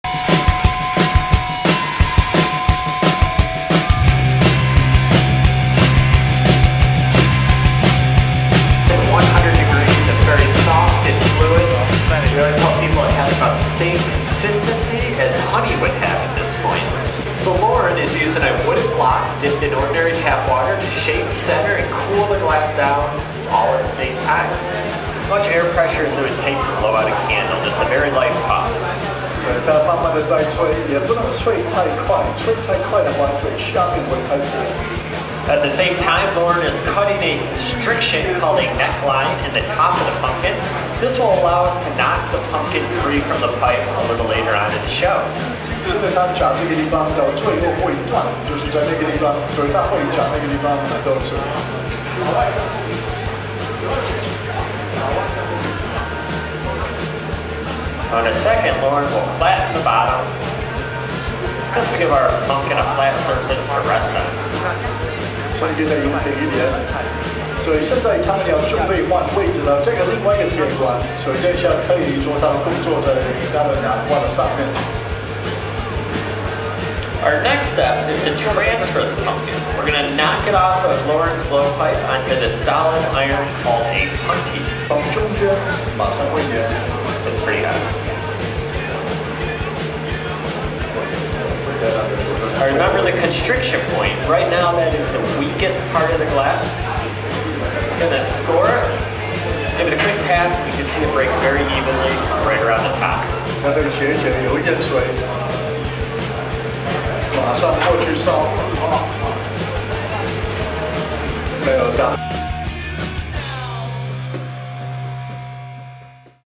Corning Glass Blowing Tutorial
Watch this impressive glass blowing tutorial straight from the Corning Glass Museum.  These guys sure know how to master glass!